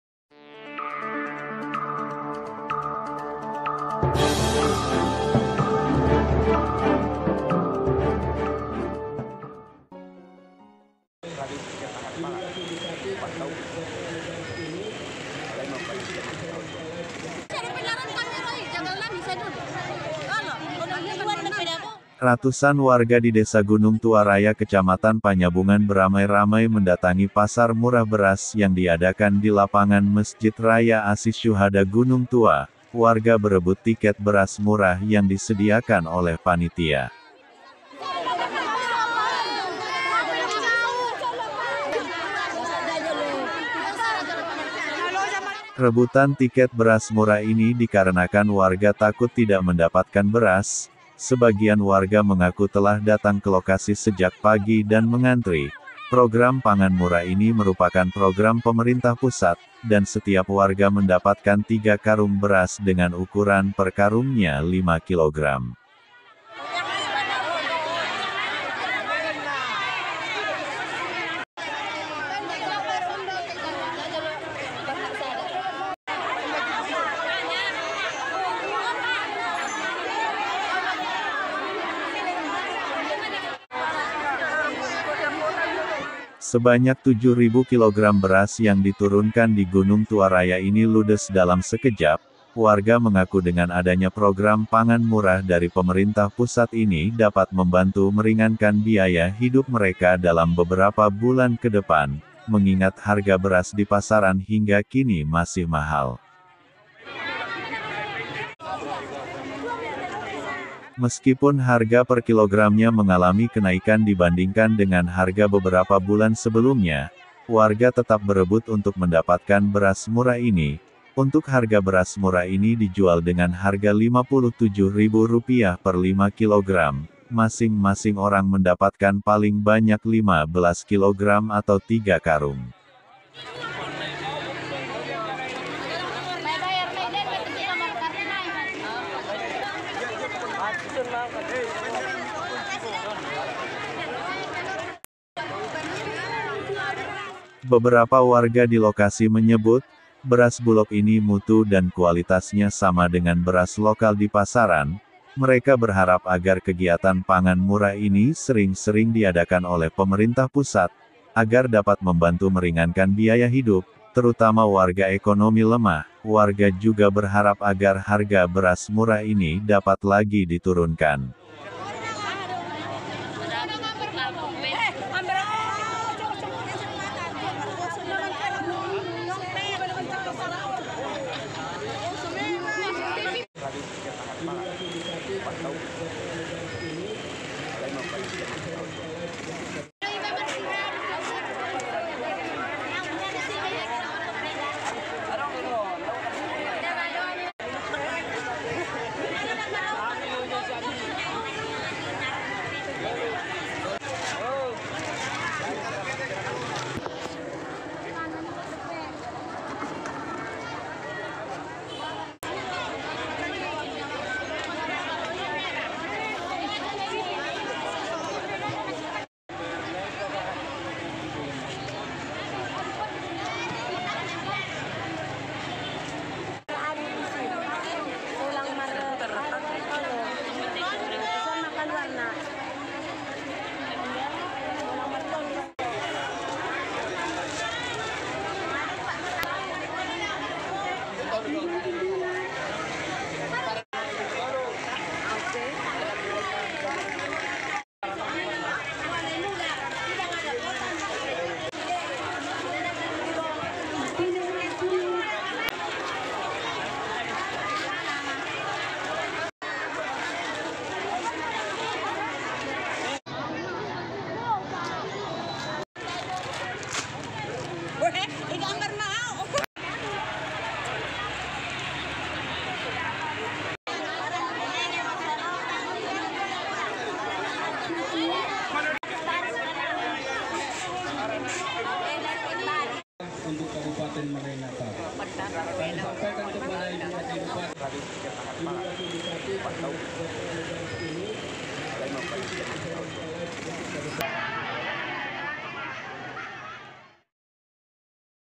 Takut tak kebagian, Warga rebutan kupon beras murah di gunung tua raya
Akibat takut tidak kebagian,warga berebut mendapatkan kupon beras murah program badan pangan nasional yang dilaksanakan di gunung tua raya, kecamatan Panyabungan Kabupaten Mandailing Natal. Sebanyak tujuh ton beras ludes dalam sekejap